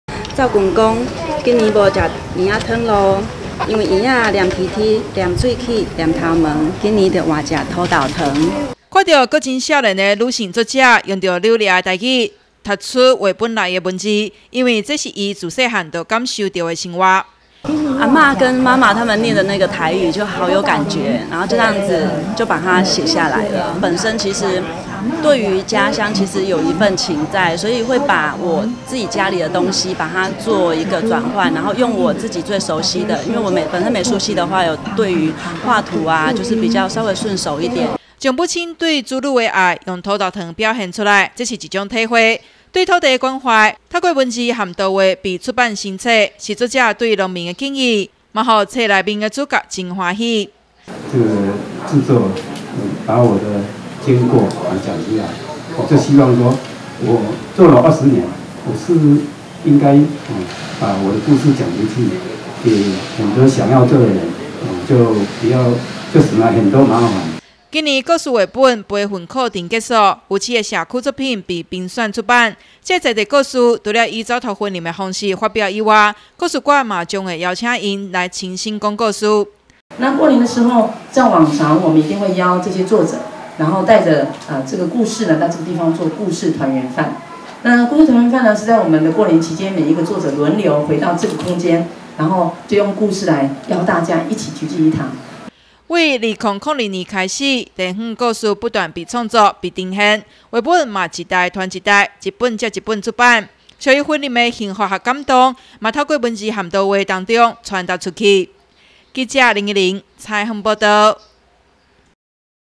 雲林新聞網-在地故事繪成冊 雲林幸福有所本
2002年，雲林故事人協會開始執行繪本創作研習，經過這麼多年，已經累積許多屬於雲林在地的故事，今年繪本培訓課程結束以後，經過評選，有七個社區作品被出版成書，裡面有地方的傳統民俗祭典，也有關懷土地、表達母親對子女關愛的故事，為了讓更多人聽到、看到這些地方創作，作者也製作故事盒，要讓大家透過各種方式，了解在這些屬於在地事。